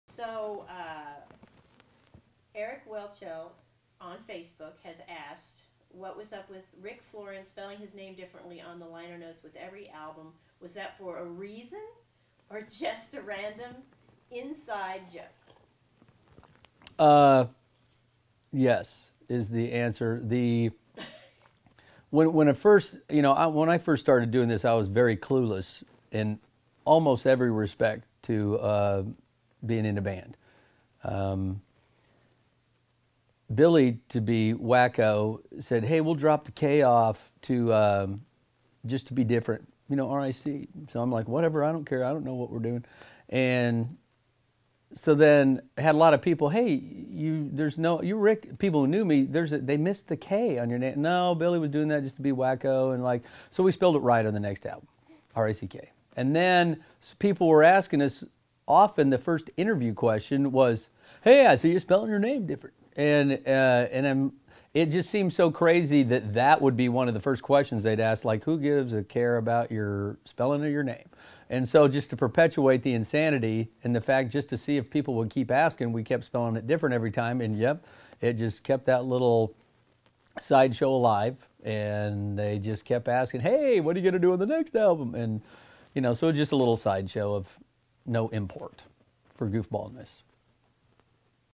Rick Rique Ric
The answer to the question: What was up with Rick Florian spelling his name differently on the liner notes of every album? Was that for a reason or just a random inside joke?